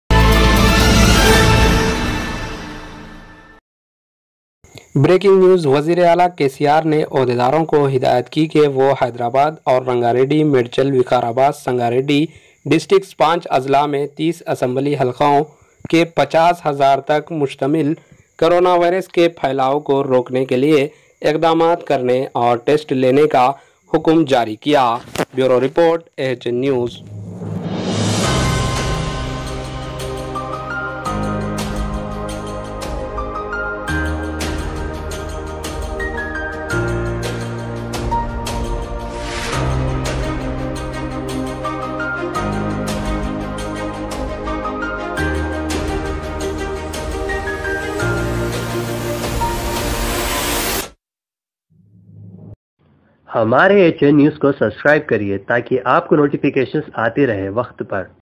Telangana: CM KCR Allows Private Hospitals, Labs To Conduct Covid-19 Tests | Hyderabad - AHN News
KCR allows private hospitals, labs to conduct COVID-19 tests; orders 50,000 tests in 5 districts

Telangana Govt set to allow private lab testing, fix COVIDー19 test price while focus on coronavirus test and containment in 30 assembly constituencies in and around Hyderabad where cases are on surge. CM KCR hold a high-level meeting,order effective fight.